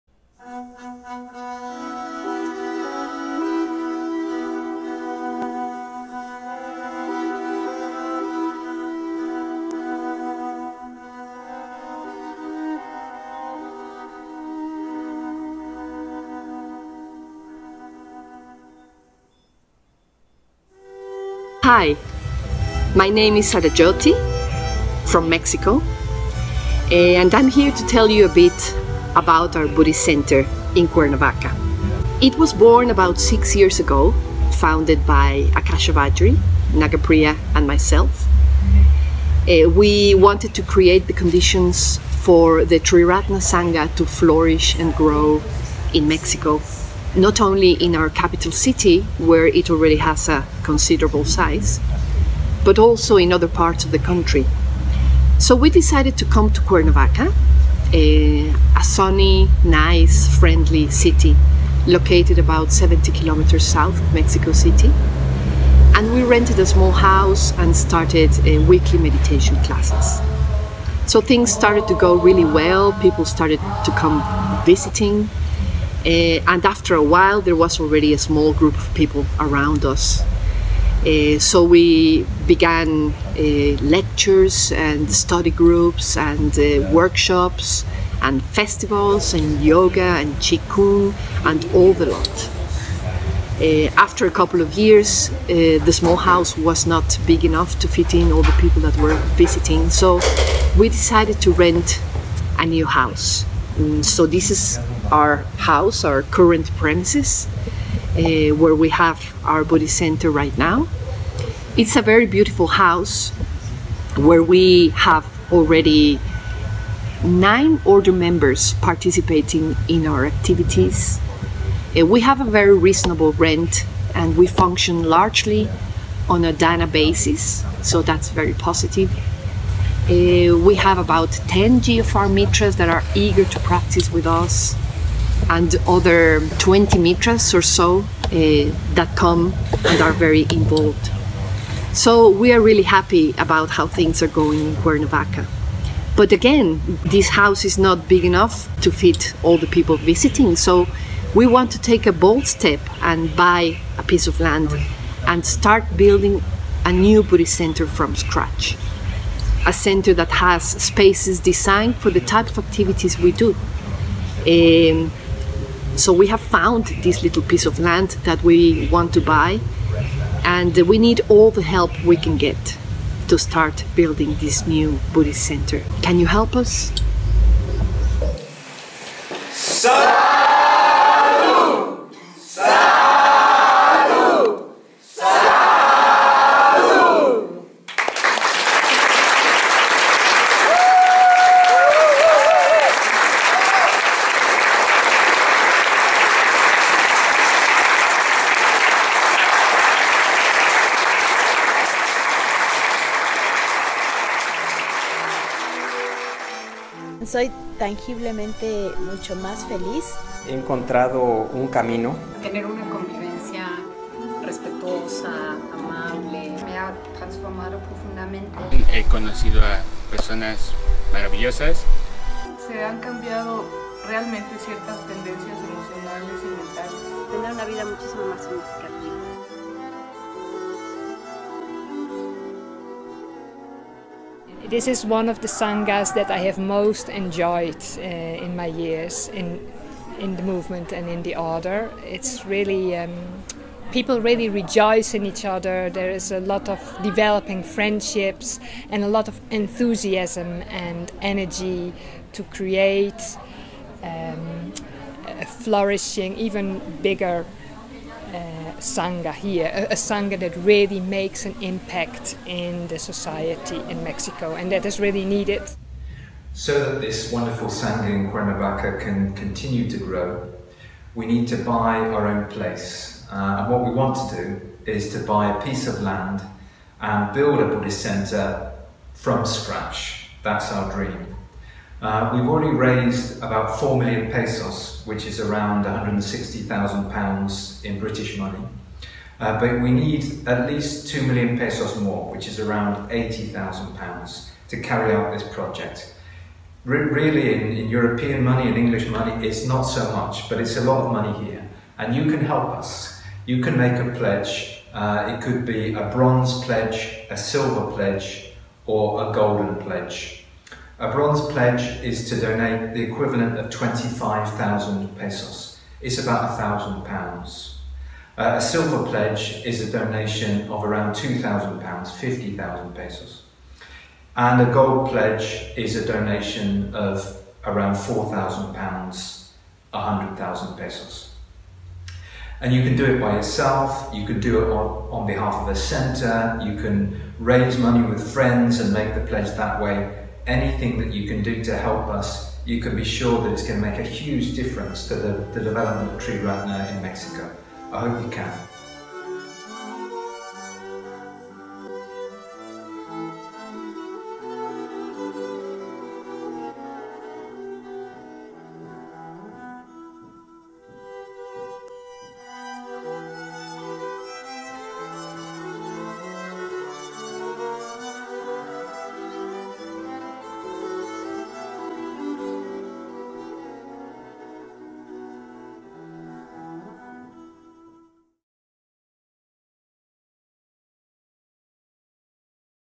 New Cuernavaca Buddhist Centre Project / Proyecto del Nuevo Centro Budista de Cuernava
Have a look of how Triratna is developing in Cuernavaca, a beautiful city in Mexico. Help us to grow even more...

Echa un vistazo de cómo Triratna está creciendo en Cuernavaca, una hermosa ciudad en Mexico. Ayúdanos a crecer un poco más...